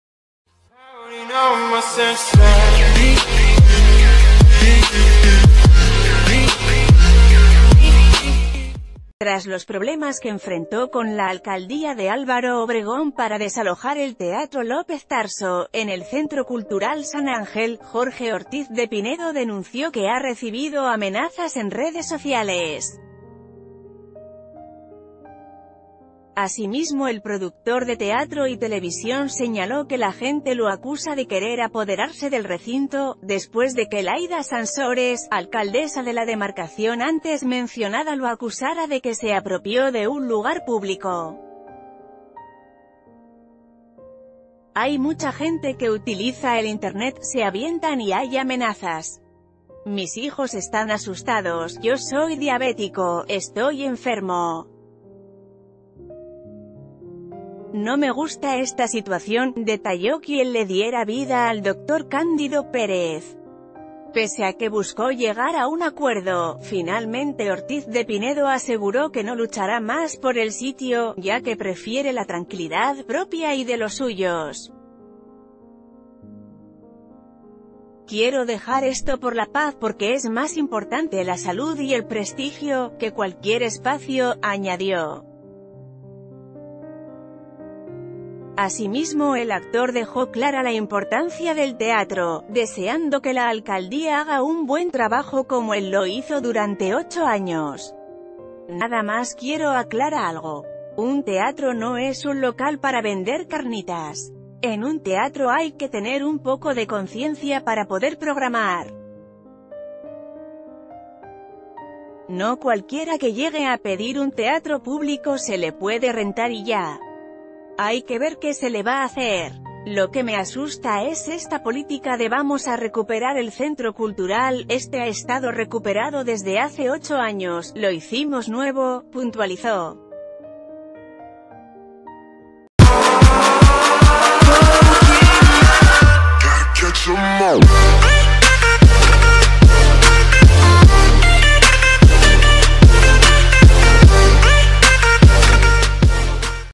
Jorge Ortiz de Pinedo denuncia amenazas
Tras los problemas que enfrentó con la Alcaldía de Álvaro Obregón para desalojar el Teatro López Tarso, en el Centro Cultural San Ángel, Jorge Ortiz de Pinedo denunció que ha recibido amenazas en redes sociales. Asimismo el productor de teatro y televisión señaló que la gente lo acusa de querer apod...